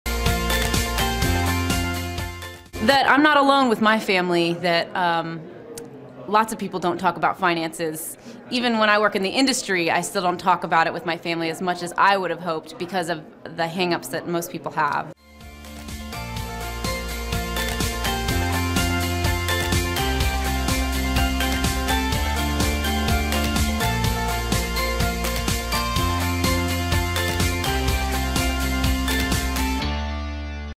Belle Schneider, VLP Advisors - What was your biggest take-away from Kathleen's keynote?
Belle Schneider, Portfolio Manager, VLP Advisors, says that her biggest take-away from Kathleen’s keynote was that she discovered that she is not alone about not openly talking about finances with family members.

Kathleen Burns Kingsbury is the founder of KBK Wealth Connection, a training and consulting firm dedicated to teaching advisors practical tips and tools for attracting and retaining women clients and their partners. She is an international speaker and author of several books including ‘How to Give Financial Advice to Women’ and ‘How to Give Financial Advice to Couples.’ Her latest book, ‘Breaking Money Silence,’ was published in 2017.